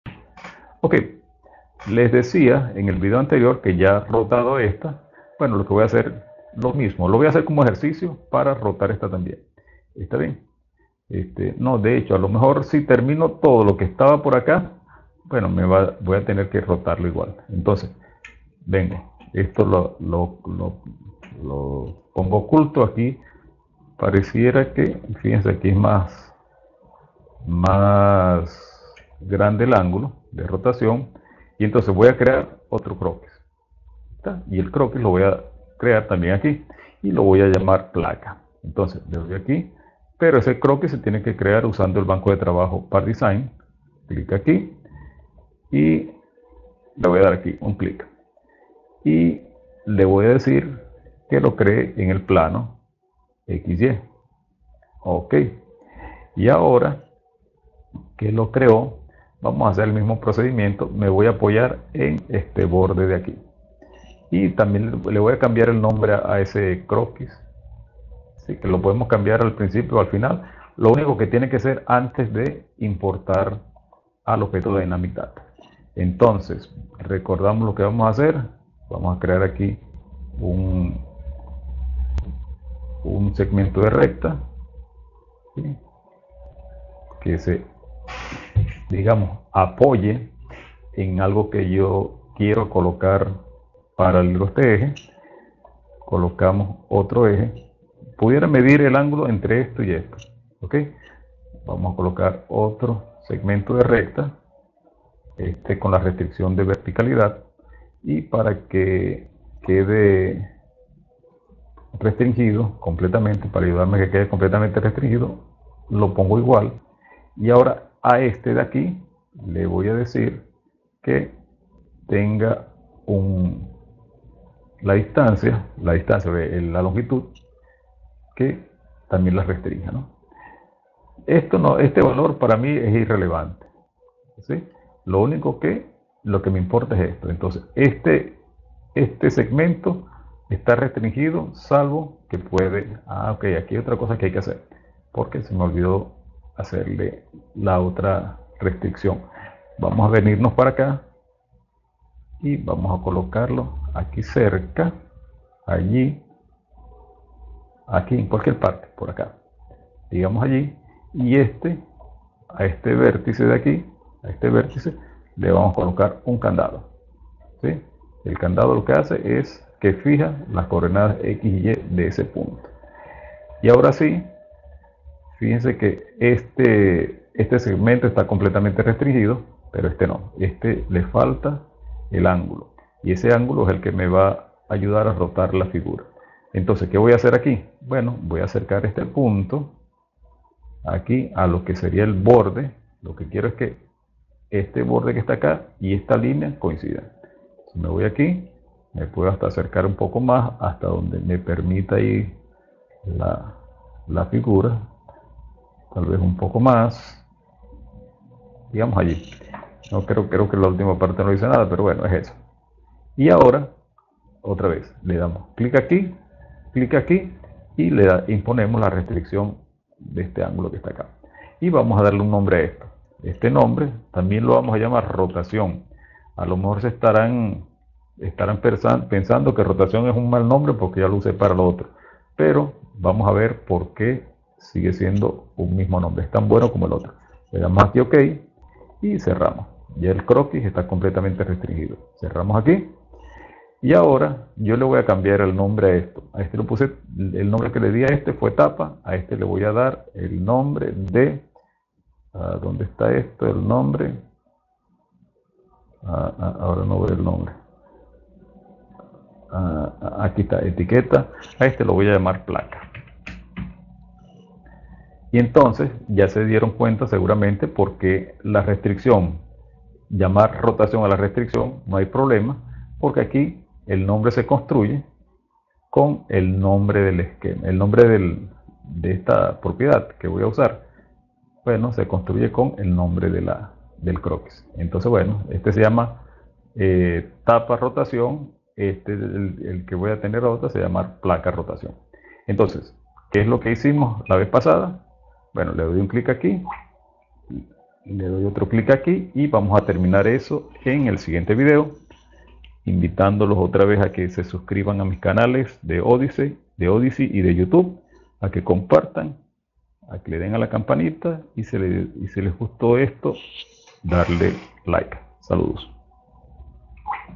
FreeCAD: Banco de trabajo Image y un flujo de trabajo(10)
Se hace una presentación sobre el uso del banco de trabajo Image y un flujo de trabajo que involucra a otros bancos.

Concretamente se diseña una pieza que luego se imprimirá en  3d para colocársela a un control remoto. El banco de trabajo Image es particularmente útil en estos casos en que se quiere hacer un poco de ingeniaría inversa.

En los primeros tres vídeos de esta serie, se hace un recorrido somero desde el inicio del proyecto hasta la impresión e instalación de la pieza. En los vídeos siguientes, se darán todos los detalles que permitirán a cualquiera reproducir lo que se ve aquí.

Aunque este proyecto es de relativa sencillez, el esquema de trabajo se puede usar en situaciones más elaboradas.